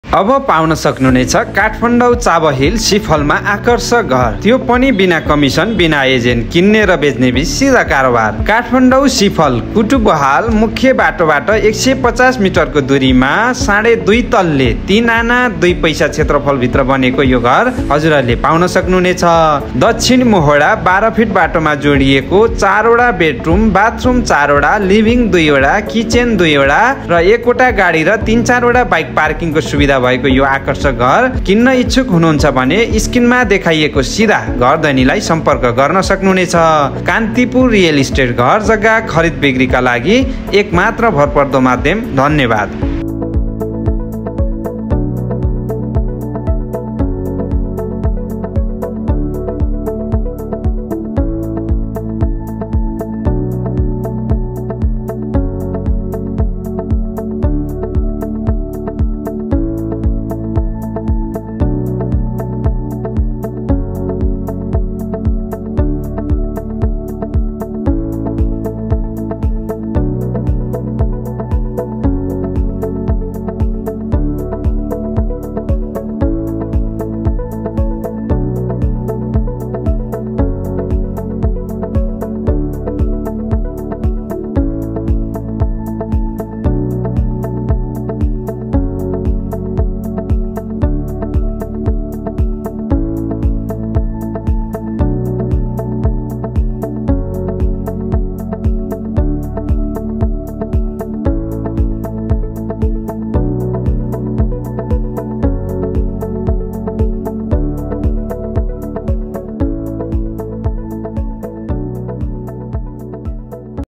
काठमाण्डौ॥चावहिल॥सिफलमा॥आकर्षक घर बिक्रिमा॥सम्पर्क नं.9851066900॥जग्गा पशुपति गुठीको रहेको छ।
काठमाण्डौ,सिफल कुटुबहाल (मुख्य बाटोबाट 150 मिटरको दुरीमा ) दक्षिण मोहोडा 12 फिटे बाटोमा जोडिएको 3 आना 2 पैसा क्षेत्रफल भित्र बनेको आकर्षक 2.5 तले घर तुरुन्त बिक्रीमा छ।यो घरको बारेमा थप जानकारी लिन चाहानुहुन्छ भने सिधा घर धनिलाई सम्पर्क गर्नुहोस्।घरधनि :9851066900
घरको बारेमा थप विवरणहरु:-
✅Bedroom:-4
✅Bathroom:-4
✅Living room:-2
✅Kitchen :-2
✅Floor:-2.5
✅Road size :-13 Feet
✅Parking:- 1 car or 3/4 bikes
 Others Facility:-
◾वास्तु शास्त्र अनुसार बनेको।
◾बिहान देखि बेलुका सम्म घाम नछेकिने चारै तिर उज्यालो भएको।
◾ढल,बत्ति,इन्टरनेट एवंम,चौबिसै घण्टा खानेपानिको सुविधा भएको।
◾सोलार सिस्टमको सुबिधा भएको।
◾Mixed,New Society,स्कुल,स्टोर,पुलिस चौकी,राम्रो रेस्ट्रो नजिकै भएको।
◾सार्बजानिक यातायातको नजिकै सुविधाभएको।
◾अण्डर ग्राउण्ड वाटर ट्रीटमेन्ट प्लान्ट र सोलार सिस्टमको सुबिधा भएको।
◾आधुनिक डिजाइन र मजबुत तरिकाबाट निर्माण भएको ।
◾स्कुल,कलेज,बैंक,सहकारी तथा हस्पिटलआदि संस्थाको नजिकै सुविधा भएको।
◾1 वटा कार 3/4 वटा बाईक पार्किङ्गको सुविधा भएको।
◾भुकम्प मापदण्ड अनुसार निर्माण गरिएको।